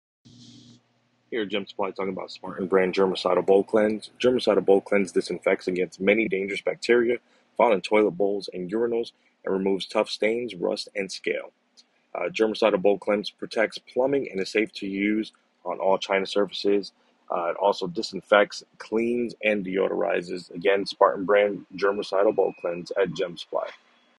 Spartan Germicidal Bowl Cleanse
Spartan Germicidal Bowl Cleanse - 12 Quart/case

Item # SP-7120-QT

Disinfects against many dangerous bacteria found in toilet bowls and urinals. Safe for use on all vitreous china surfaces.

12/32 oz. per case, includes swab mop and gloves.
Qt., 12 quarts/case